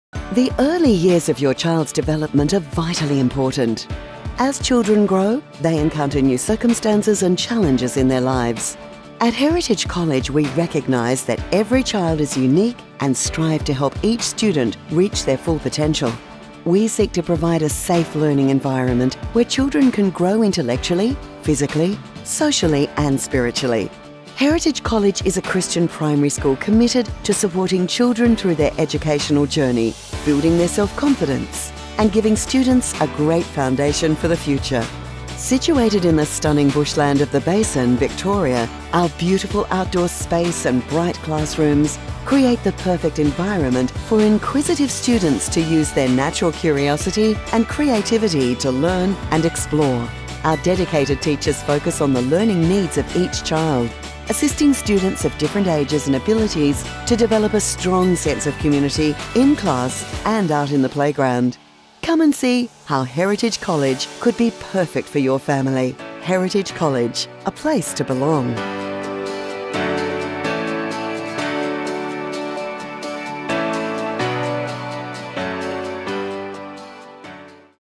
Heritage College Intro
Discover the beautiful, godly environment your children can enjoy at Heritage College in The Basin.